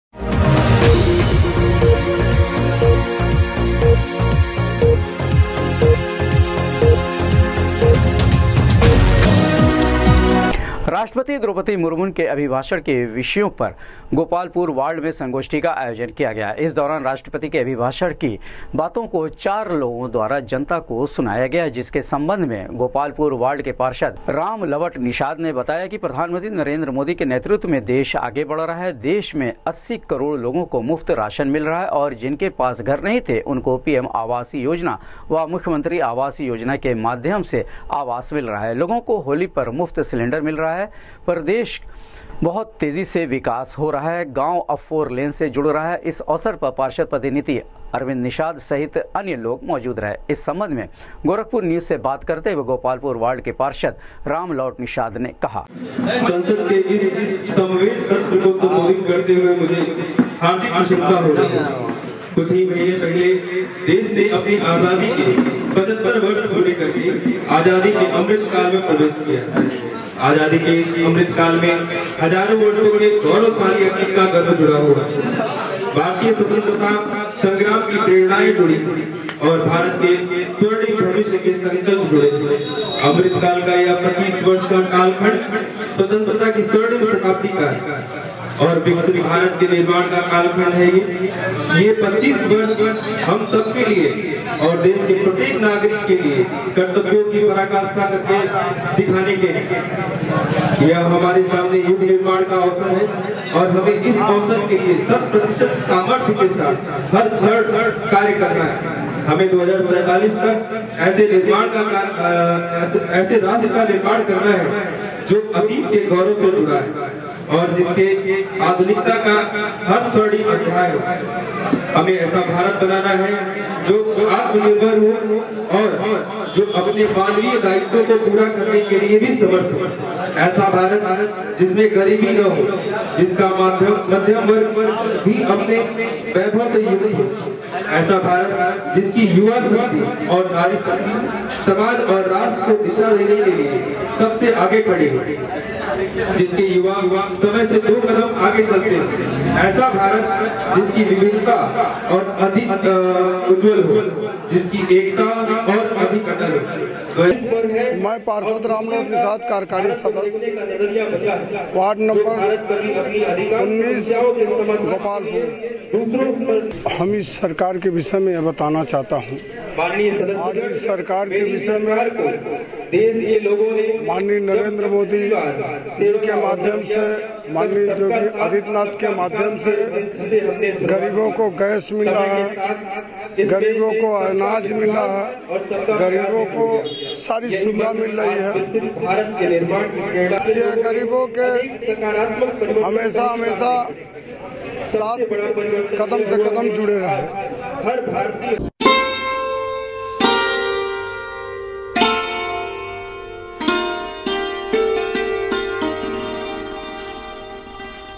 गोपालपुर वार्ड में राष्ट्रपति द्रौपदी मुर्मू के अभिभाषण के विषयों पर संगोष्ठी का किया गया आयोजन
गोपालपुर वार्ड में राष्ट्रपति द्रौपदी मुर्मू के अभिभाषण के विषयों पर संगोष्ठी का किया गया आयोजन,चार नागरिकों  द्वारा राष्ट्रपति के द्वारा संसद में दिये गये अभिभाषण को पढ़ कर वार्ड वासियो को गया सुनाया..